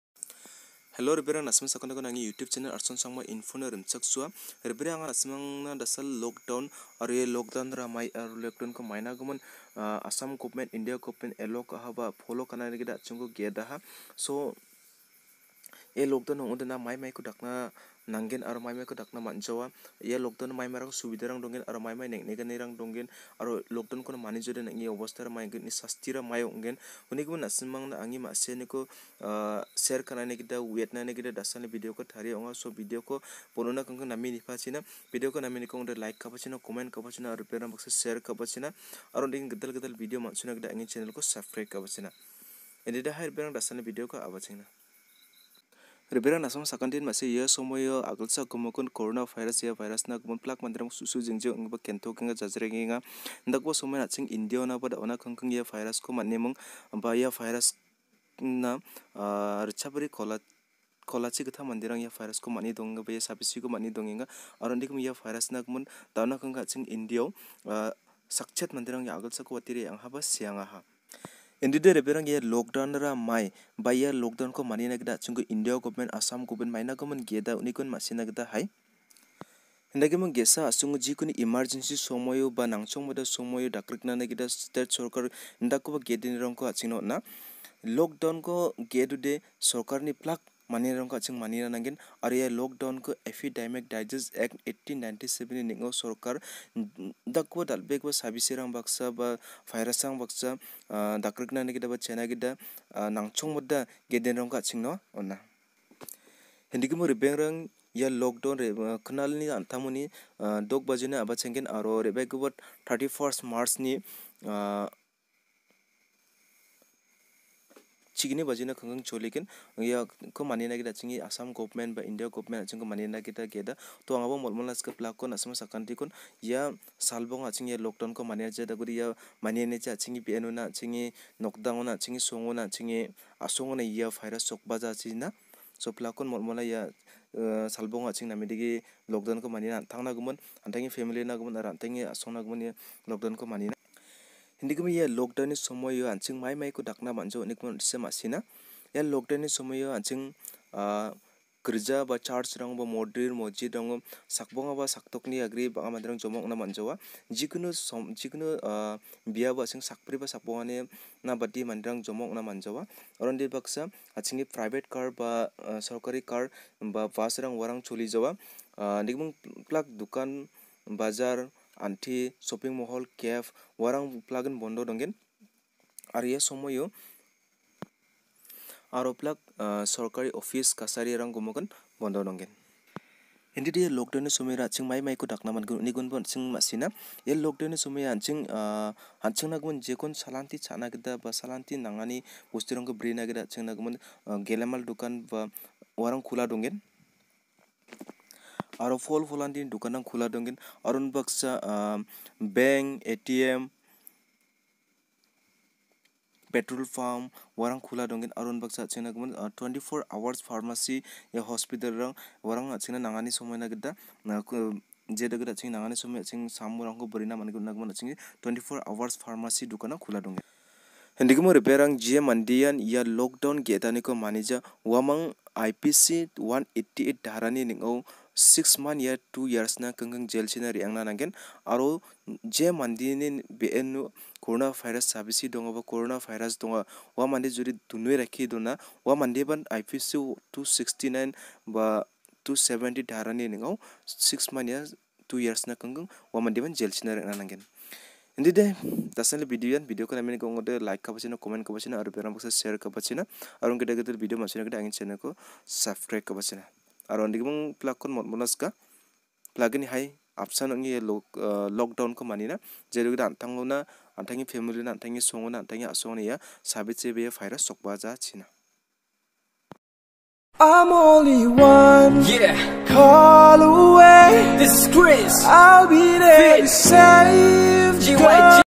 Assam Lockdown|| 24th March 6pm to 31st March 12pm|| Arson Sangma info
Assam Lockdown||
24th March 6pm to 31st March 12pm

Manijaode 2years na jail reangna naggen.

pilakon manina gita molmolenga.
antangni nokoni ongkatna be, nagchongmota kamnasa apalo ongkatbo.